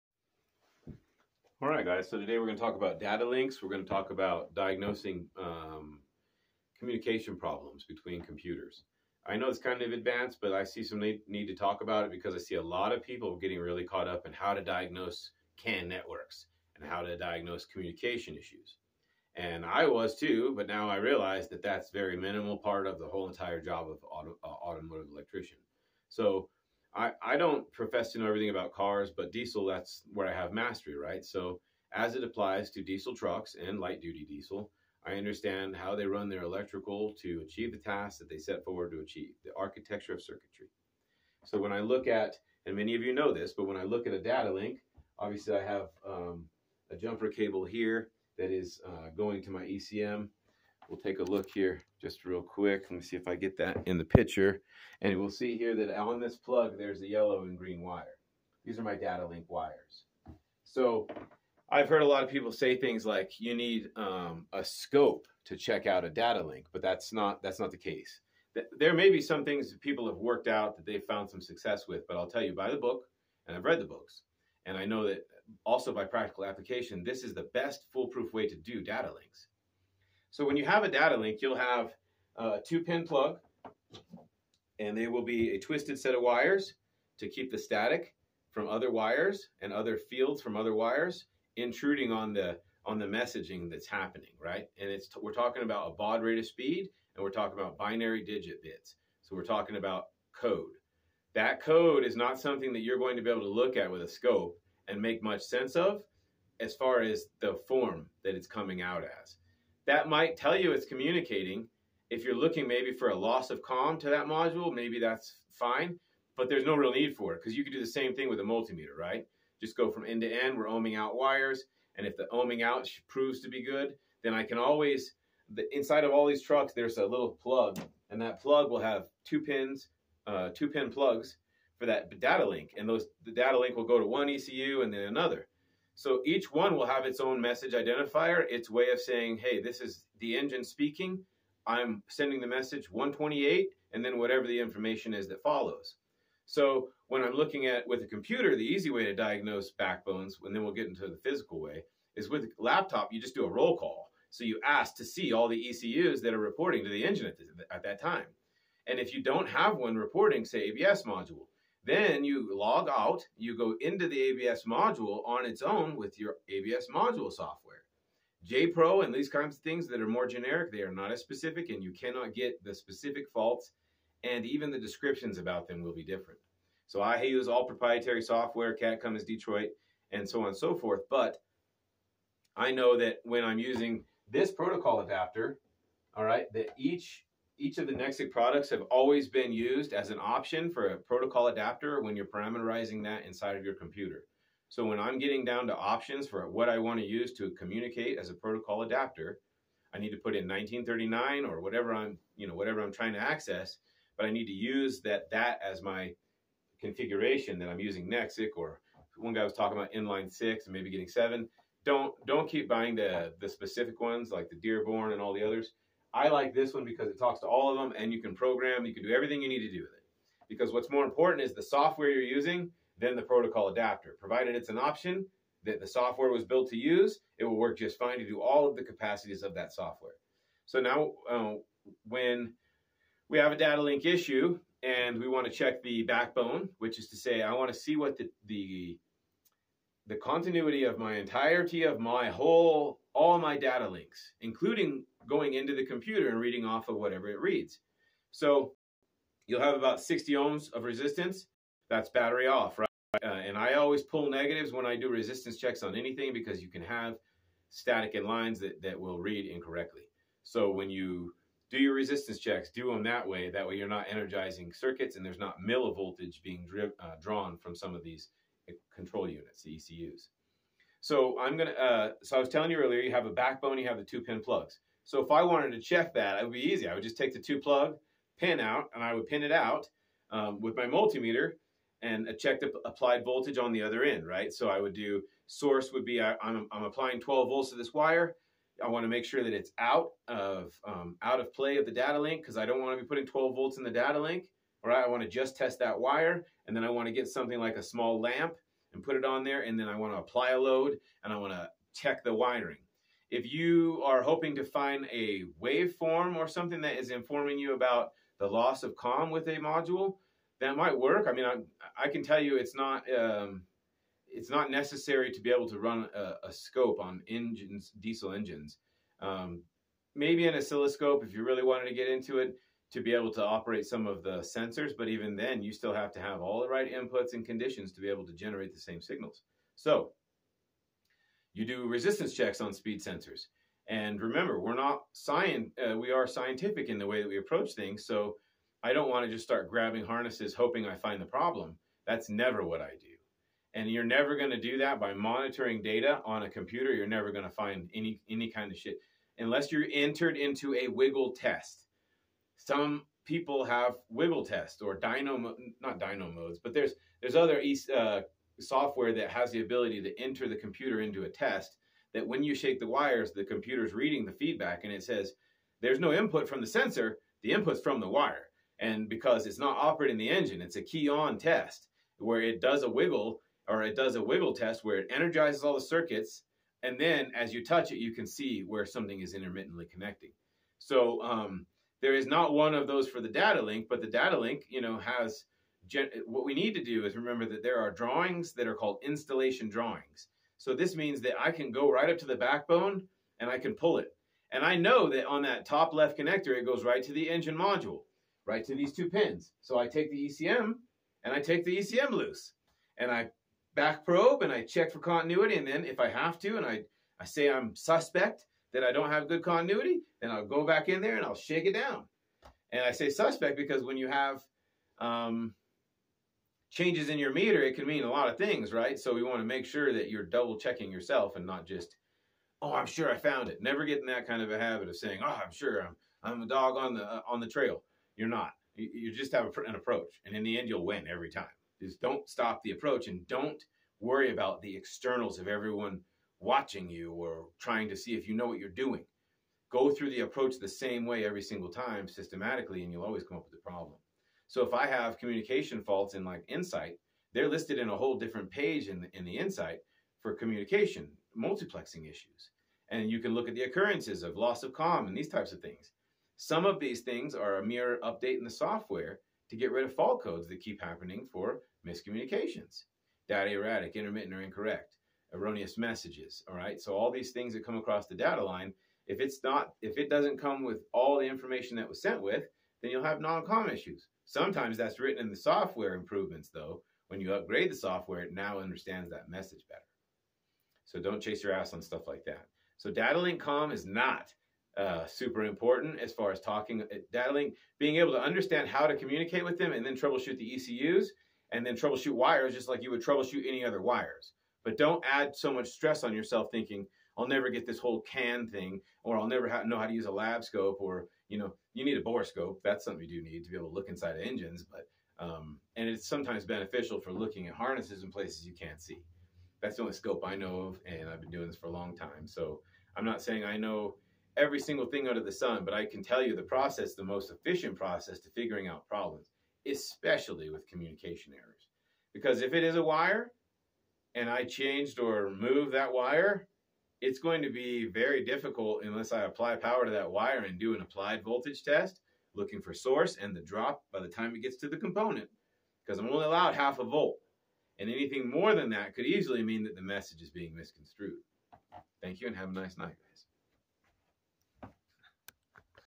Datalink diagnostics. how to diagnose comm and ecu comm fault issues on big trucks.
Industrial diesel is built differently than automotive. here is how. I have much more to share about this, but it won't be till the basics are covered.